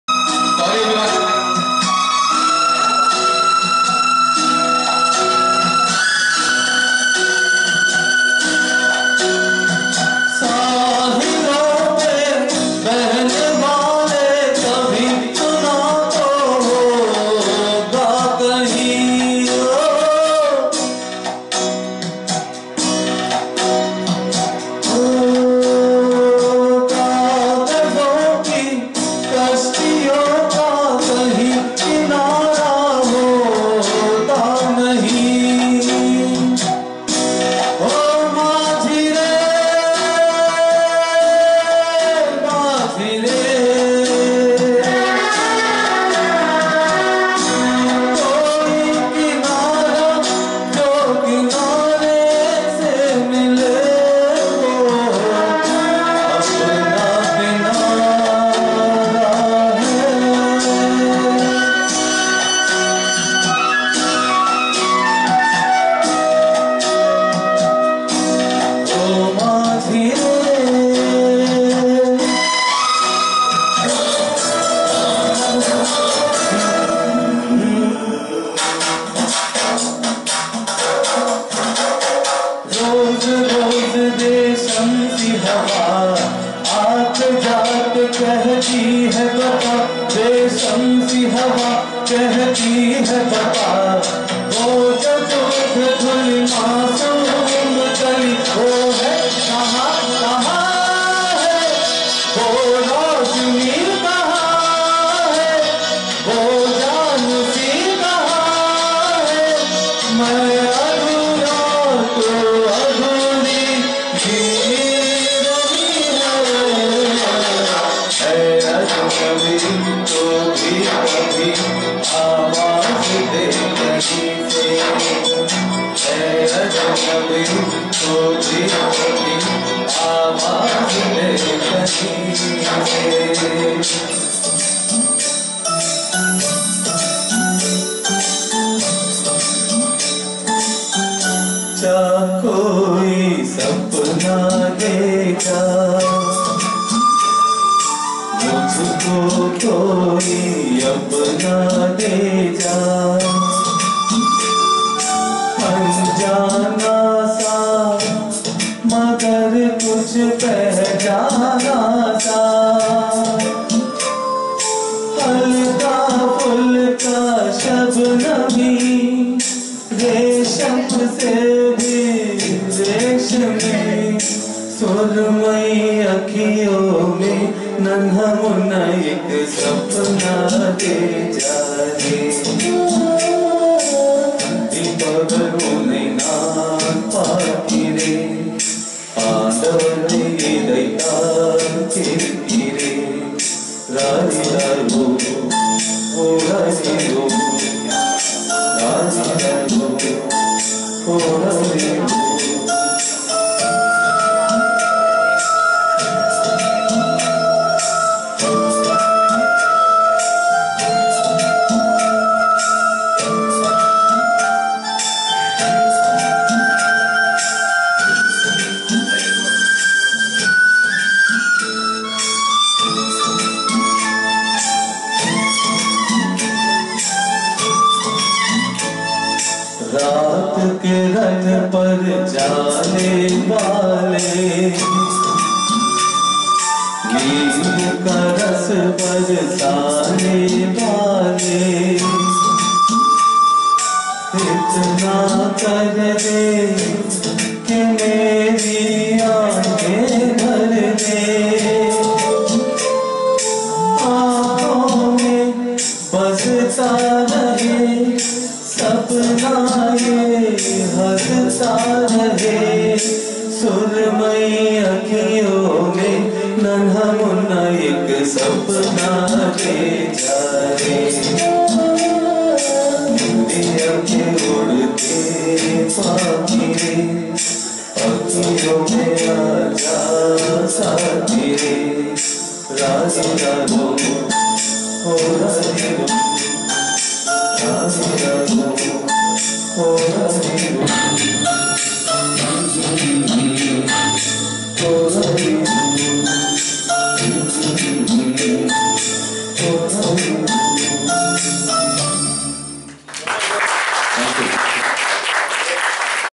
Vivek S Medley
The Creative Indian and Co - Sargam show - Gulzar Theme.
Sampooran Singh Kalra (born 18 August 1934), known professionally as Gulzar or Gulzar Saab , is an Indian lyricist, poet, author, screenwriter, and film director